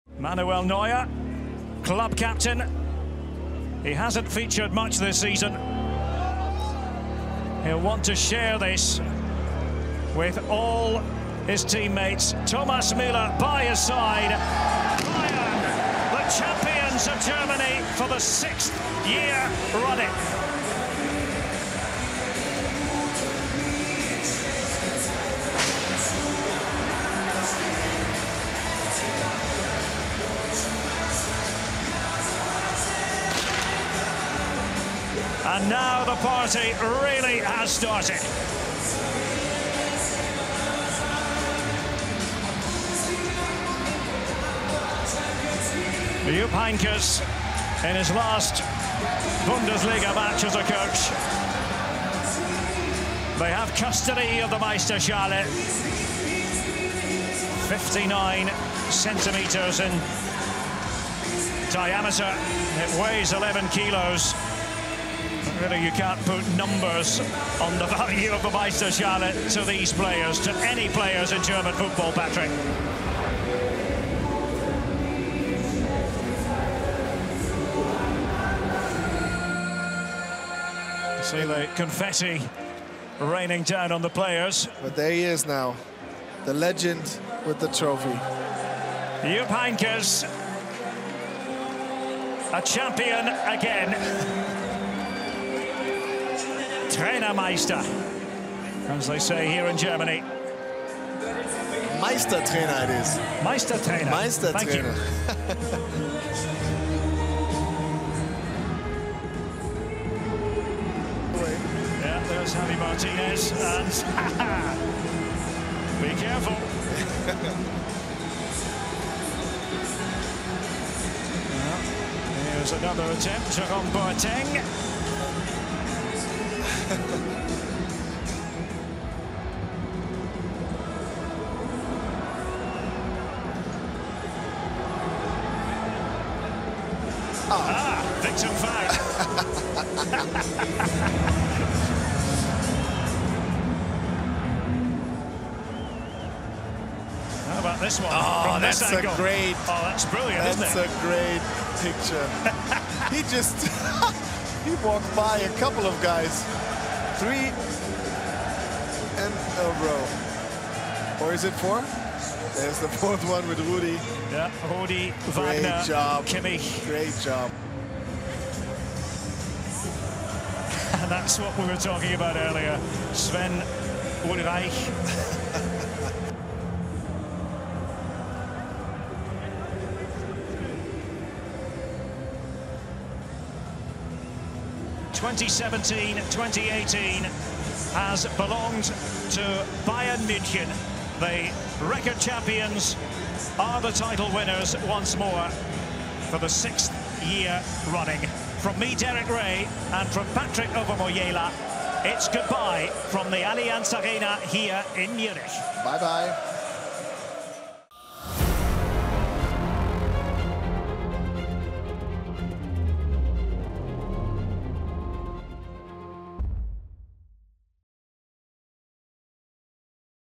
Trophy Lifting! - FC Bayern München Lift The Meisterschale - Heynckes Has The Honour
Bayern München are presented with the trophy

They’ve done it! FC Bayern München are Bundesliga champions for the sixth year running. And it was the final time that coach Jupp Heynckes held the trophy aloft. Cue the celebrations at the Allianz Arena in Munich.